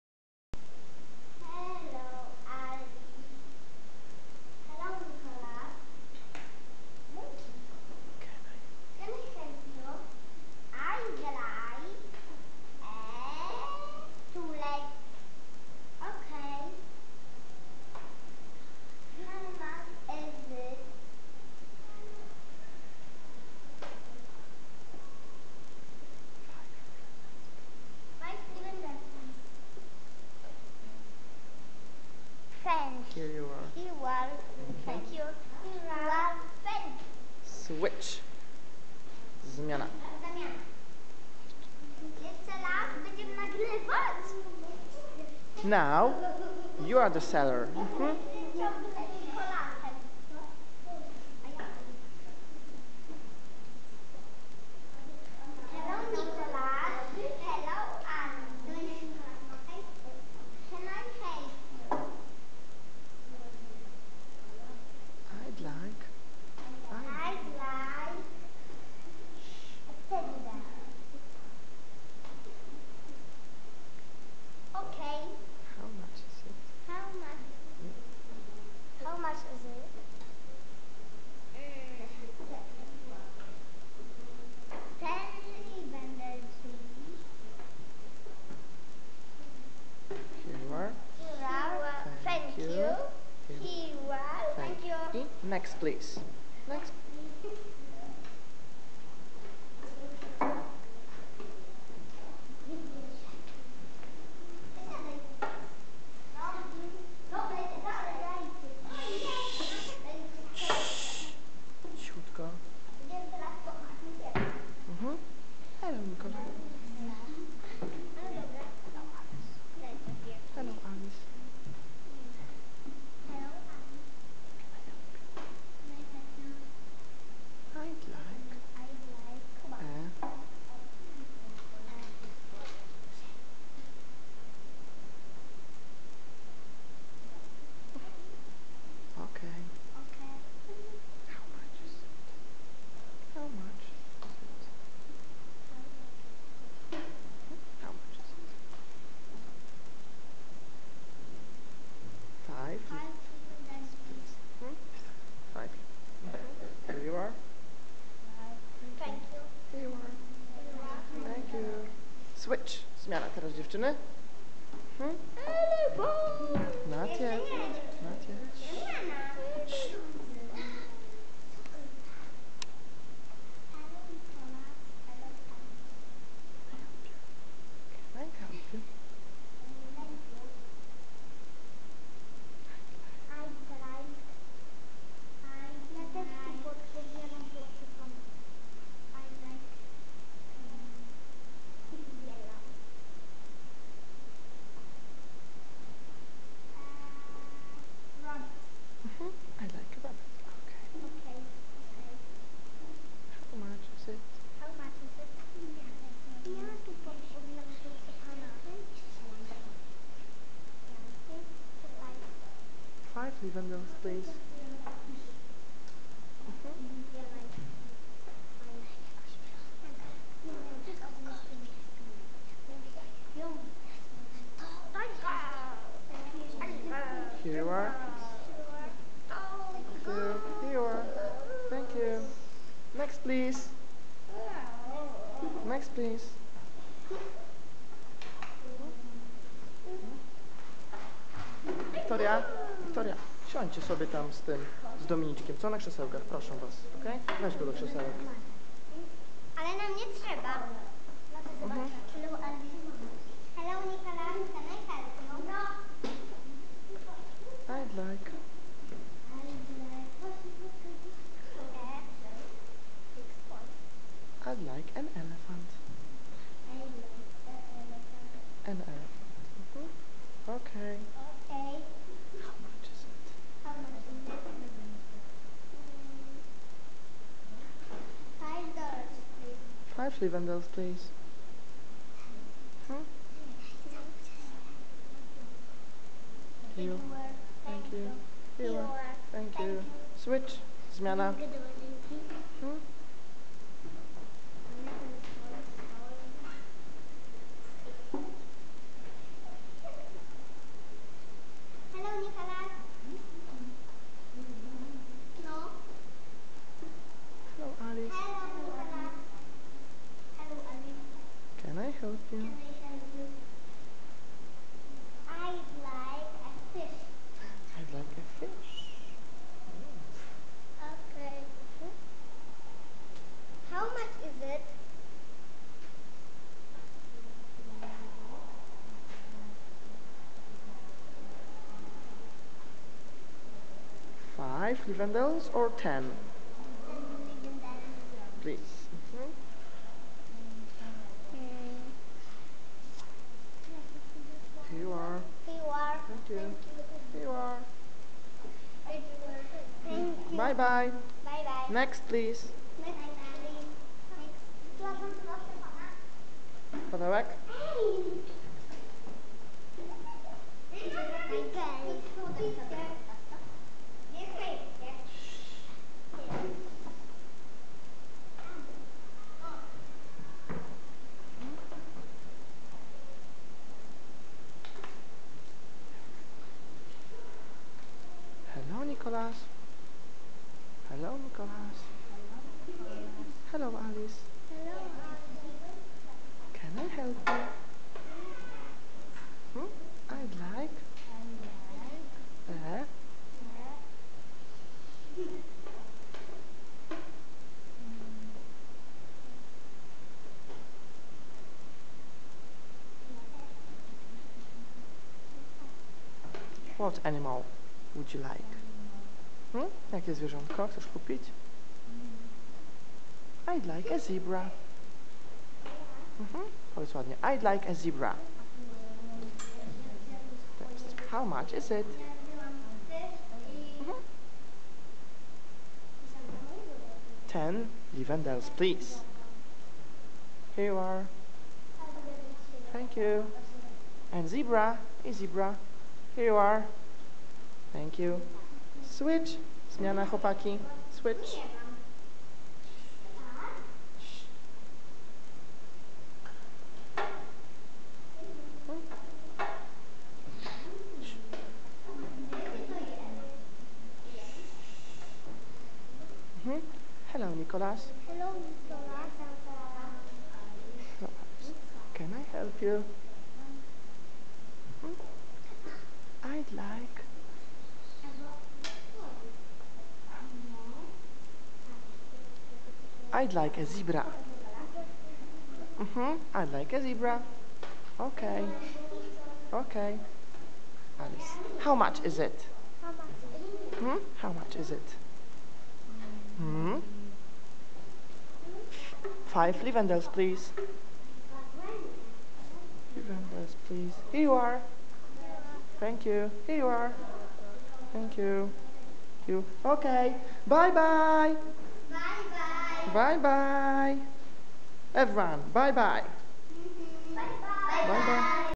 NLS 3 BLABBERS' PET SHOP - dialog w sklepie zoologicznym (szkoła partnerska)
Dzieci odgrywają scenkę sytuacyjną w sklepie zoologicznym. Dzięki nauce dialogów z realnych życiowych sytuacji uczą się codziennej komunikacji w języku obcym. W metodzie MAŁA LINGUA duży nacisk kładziemy właśnie na mówienie w języku obcym.
Samodzielnie wykonane banknoty i przyniesione rekwizyty pomagają poczuć się pewnie i pozwalają dzieciom identyfikować się z daną sytuacją.